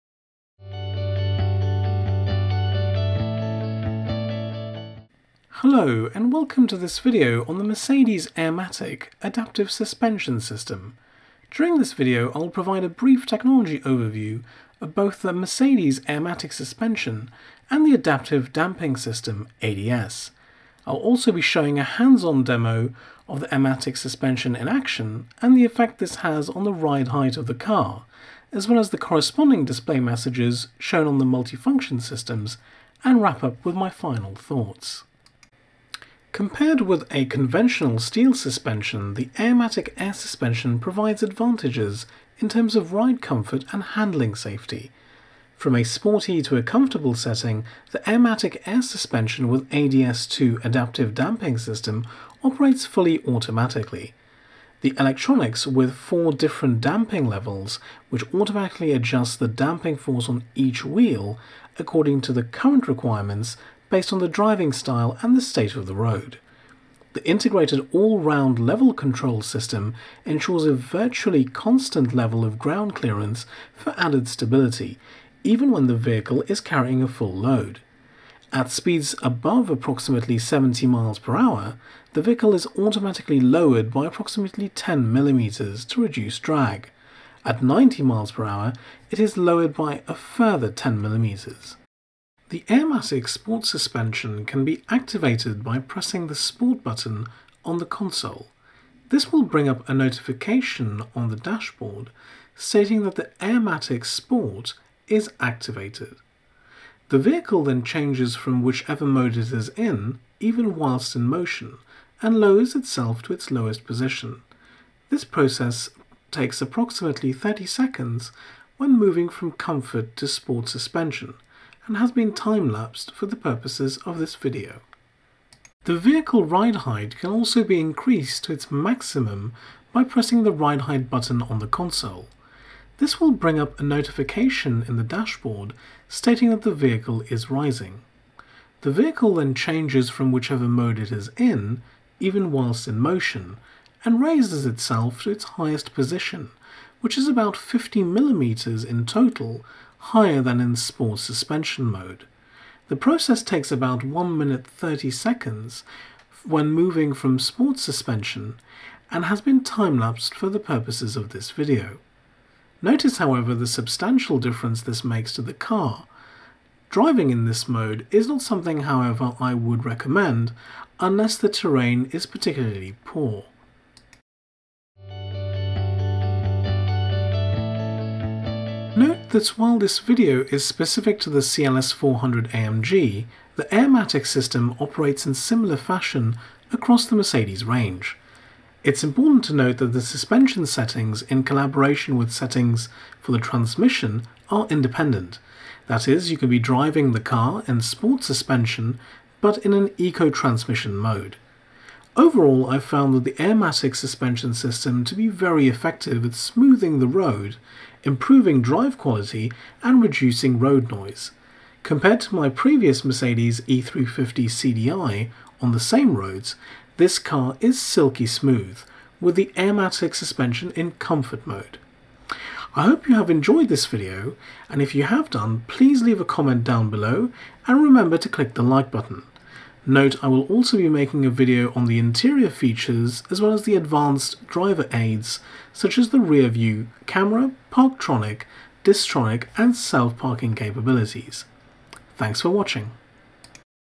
Mercedes AIRMATIC Air Suspension Hands on Review
The Mercedes-Benz AIRMATIC air suspension and the Adaptive Damping System (ADS) provide noticeable advantages in terms of ride comfort, handling and drive safety when compared to a typical steel suspension.

During the video I will also be showing a hands-on demo of the AIRMATIC suspension in action and the effect this has on the ride height of the car as well as the corresponding display messages shown on the multifunction systems and wrap up with my final thoughts.